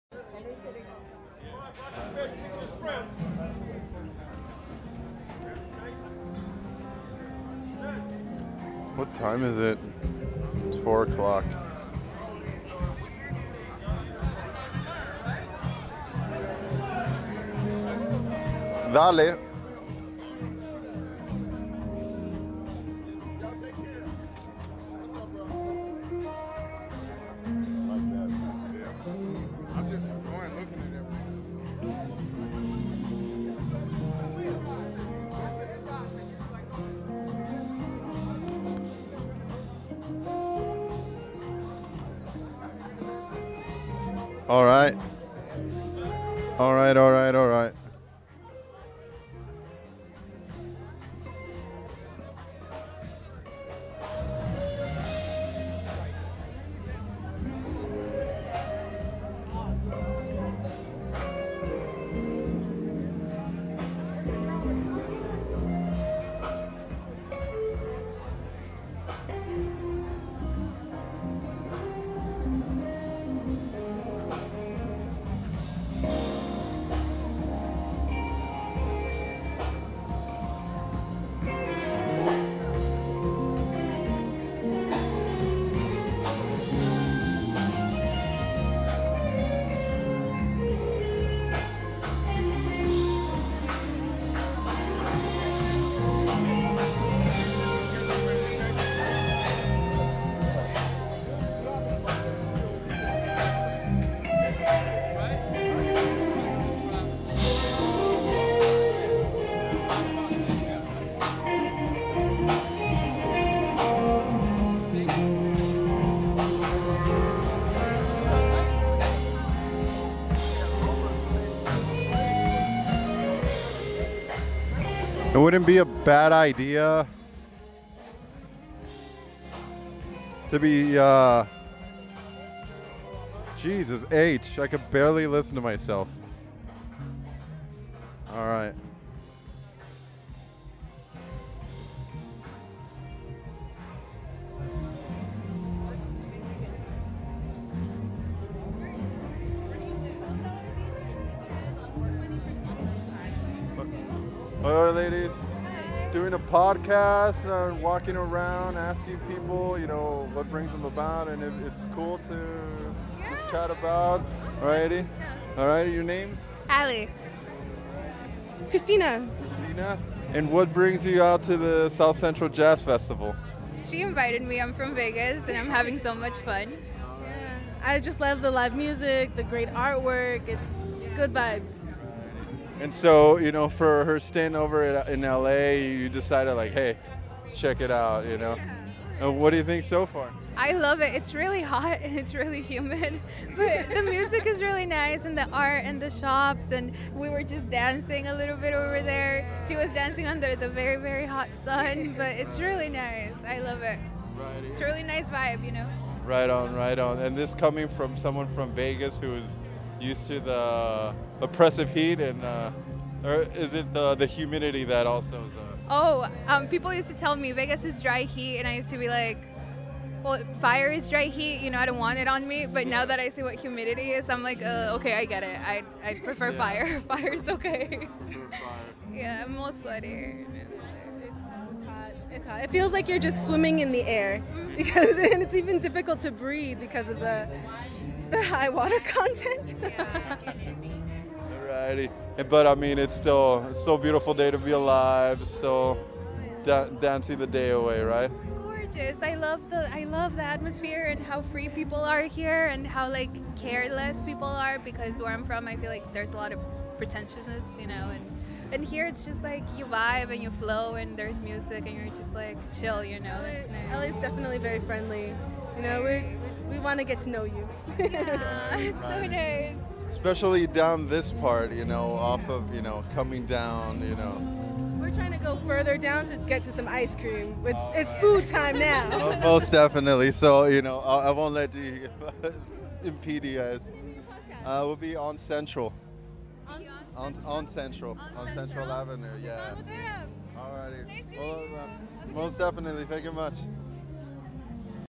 Central Ave. Jazz Festival 2019 (Part 9)
This was a prototype for a podcast I had in mind. As you can tell it’s been a minute from the day it was recorded till publication, but the vibe is there. Angelinos (and visitors) grooving along a hot Saturday afternoon in July.

P.s. Yes, I am aware that I keep referring to the festival as “The South Central Jazz Festival” instead of its given name of “Central Ave. Jazz Festival.”

But hey, ¯\_(ツ)_/¯.

Around these parts we still call it “South Central LA” instead of the insipid moniker of “South LA”.